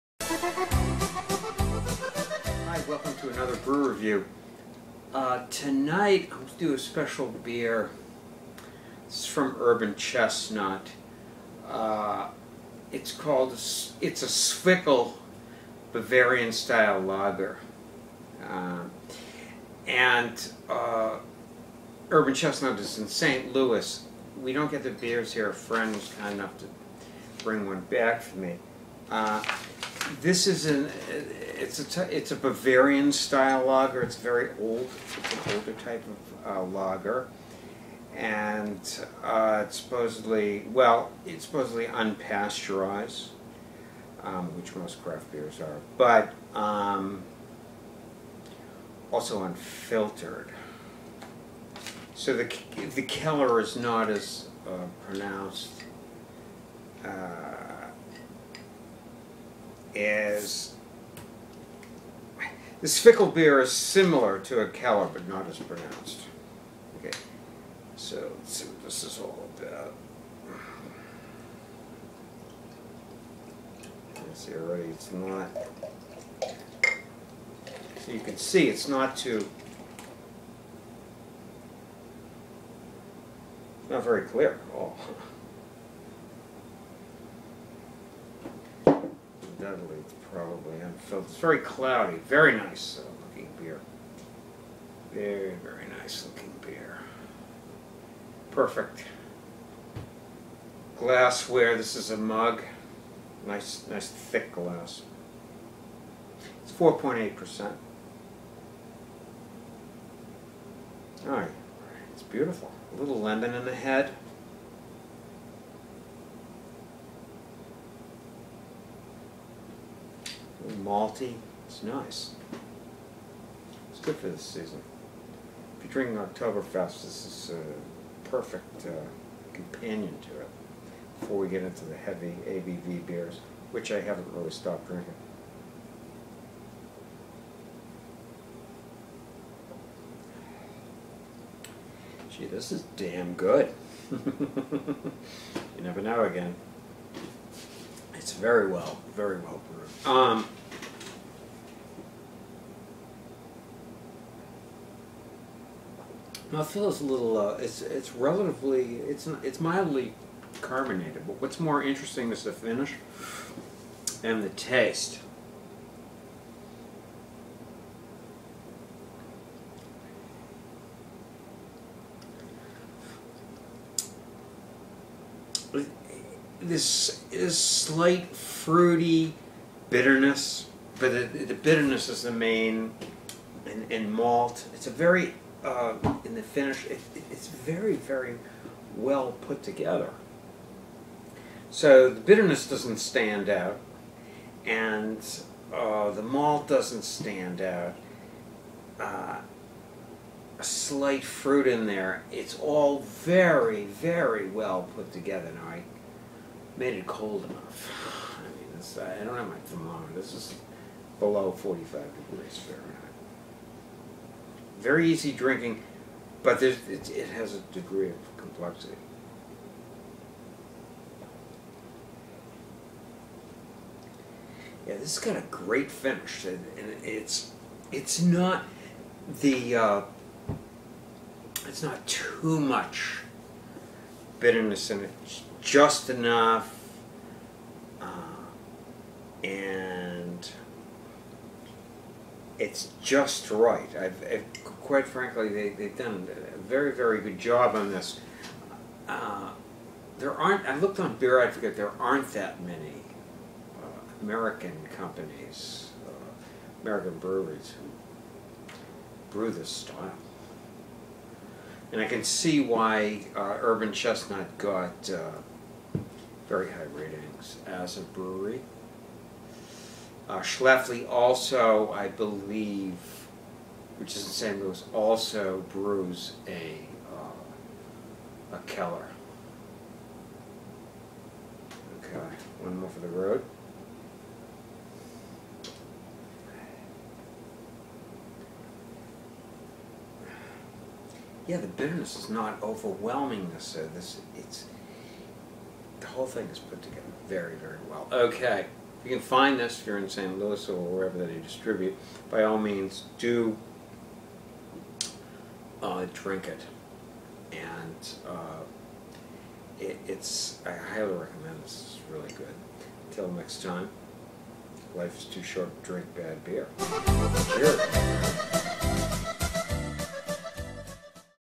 Zwickel Bavarian Style Lager: Urban Chestnut Brewing Company St. Louis 4.8%ABV
The Zwickel Bavarian Style lager from Urban Chestnut is an excellent balance of malt, fruit, and bitterness. Crisp, and especially enjoyable during the Autumn.